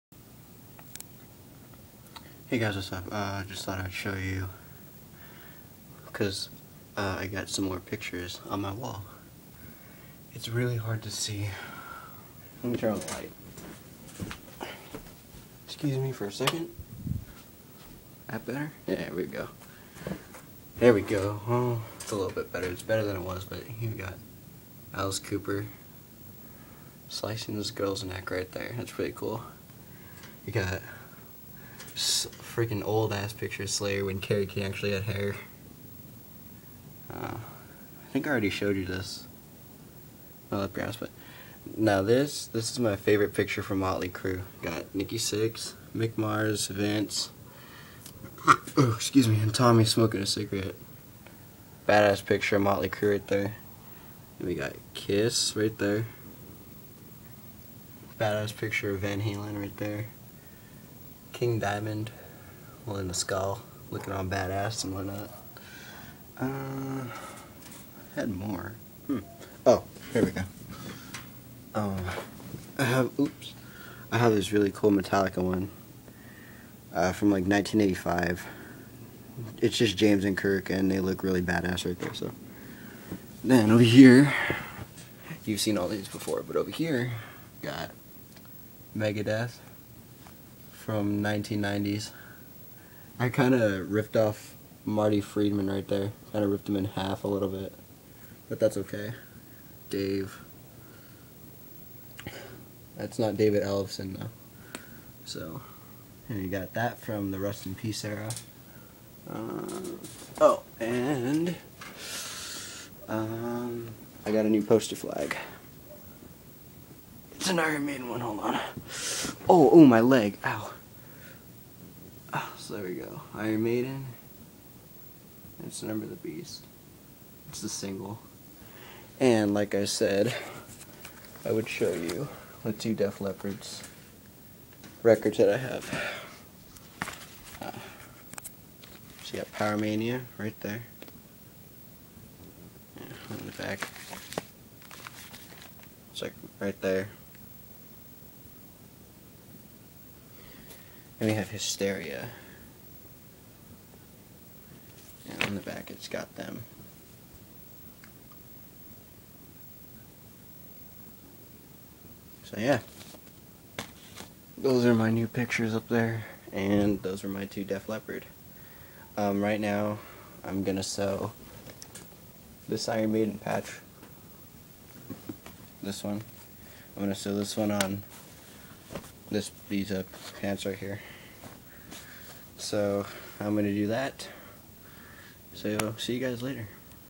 New Pics in My Room/Def Leppard Records
My new pics and my two def leppard vinyls both are ORIGINAL(: